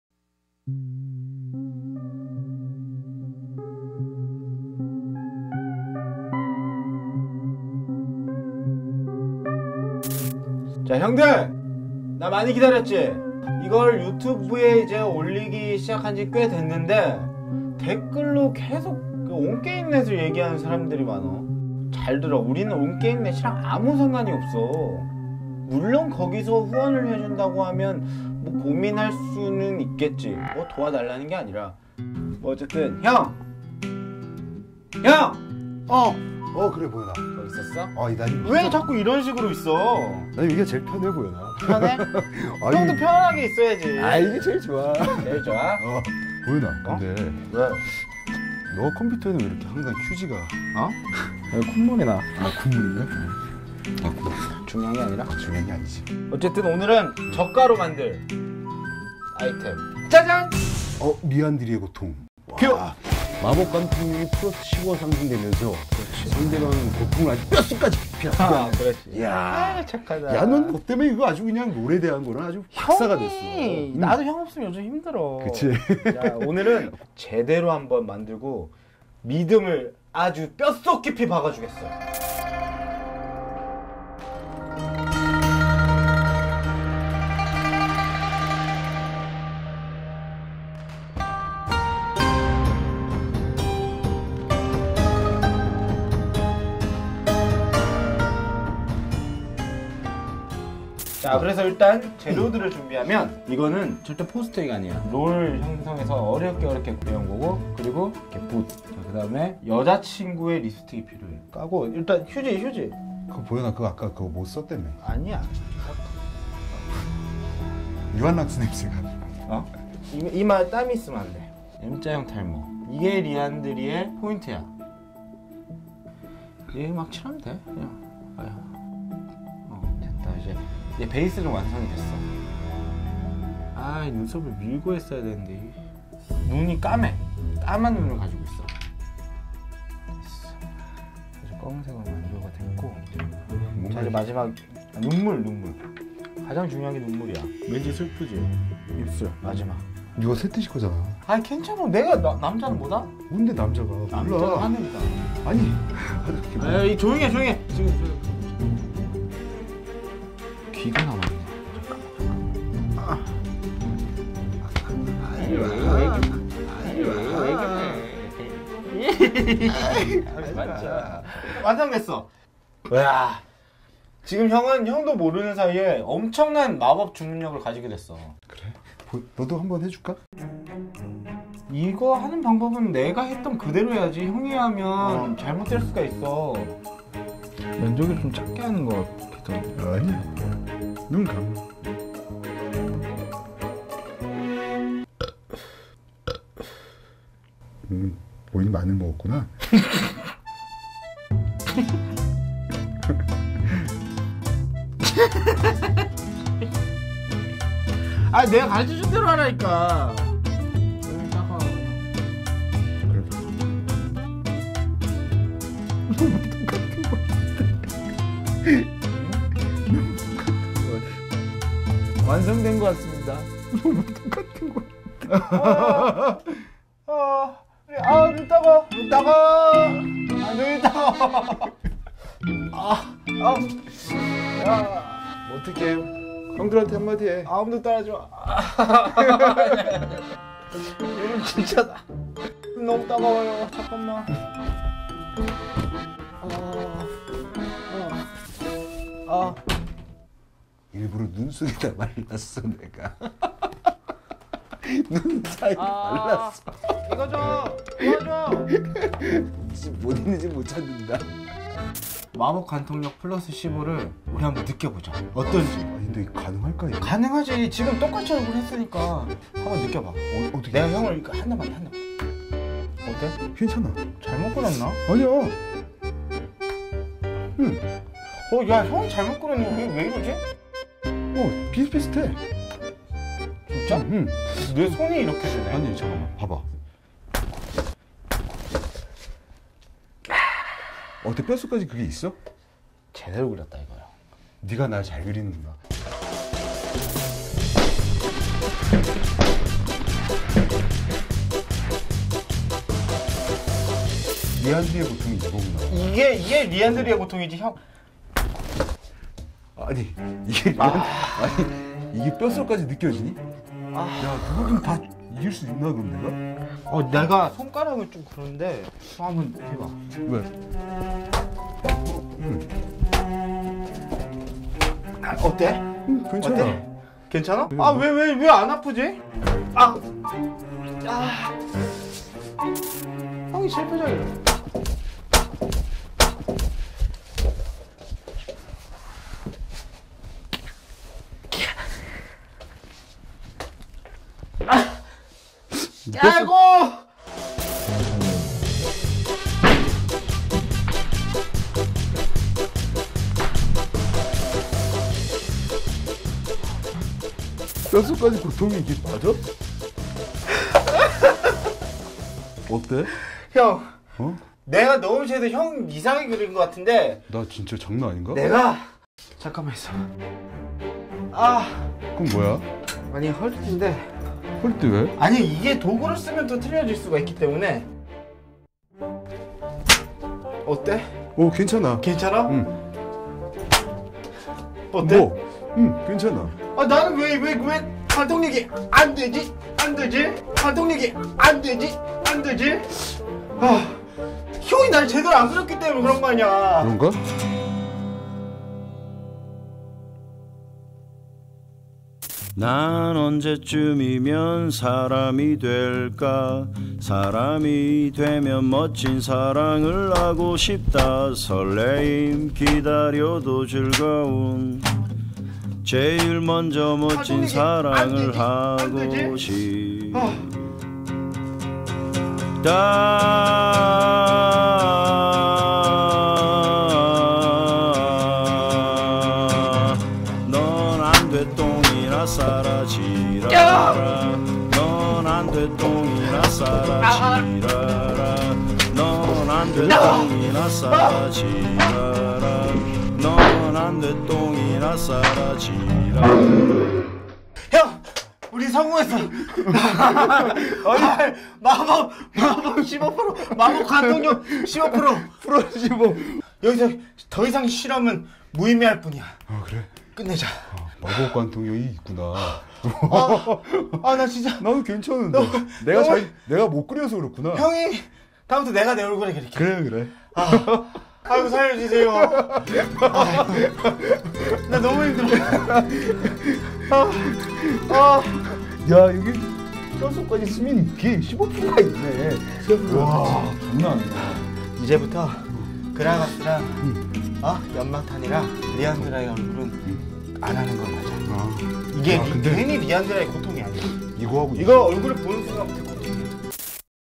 롤리터 5화 실제 재현 쇼~!(리안드리 가면) LOLiter: League of Legend Reality Show (EP5)
형들안녕?!
오늘 증명할 아이템은 리안드리의 고통을 준비했어~

마법관통력 +15라는 엄청난 아이템이지!!

준비물

1. 붓
2. 페이스 페이팅
3. 립스틱

리안드리의 고통은 디테일이 살아야 비로소 마법관통력이 생기는거 잊지말라고!!

롤은 뭐다?! 진짜다!!

What's up bros?!
Today item is the Liandry's Torment~

Magic penetration +15, this is a f* awesome item.

Recipe

1. brush
2. face painting
3. Lipstick

The important thing to make the Liandry's Torment is
the DETAIL expression for its magic penetration.

What is LOL? It is REAL!!